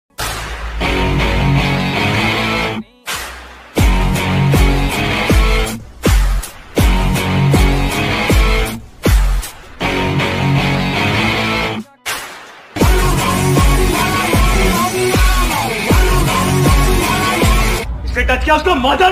Moto Vlog. Chappari Moment 3.
Hello Guys.
Caught another chappari rider while Moto Vlogging.